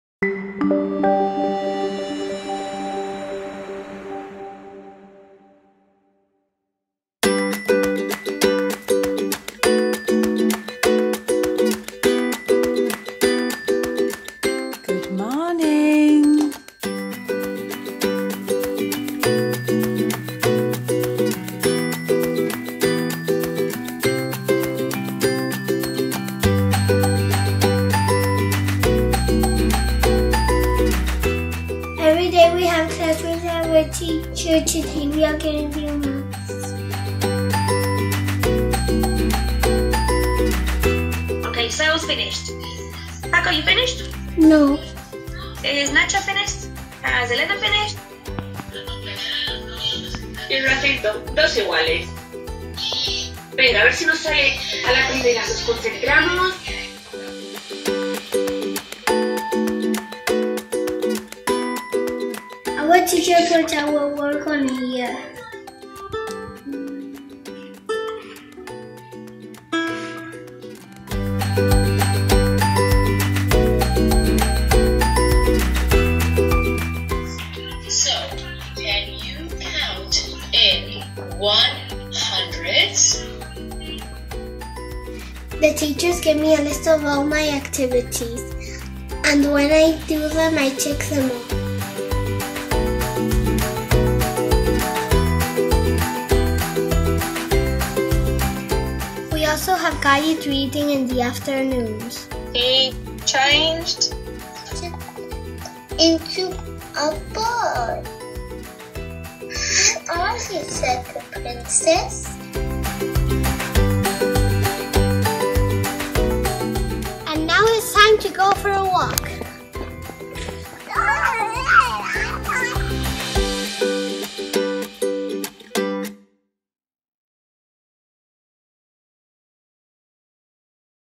Online teaching & learning - Early Years - ELIS Villamartín
At ELIS Villamartín we have been able to adapt rapidly and successfully to a new situation of online teaching and learning, providing students and teachers with the necessary tools and resources to ensure rich online learning experiences and to help them stay connected.
Live the experience from our students’ perspective!

En ELIS Villamartín hemos sido capaces de adaptarnos con rapidez y éxito a una nueva situación de enseñanza y aprendizaje online, proporcionando a los alumnos y a los profesores las herramientas y los recursos necesarios para asegurar una gran experiencia de aprendizaje a distancia y para ayudarles a seguir en contacto.
¡Vive la experiencia desde la perspectiva de nuestros alumnos!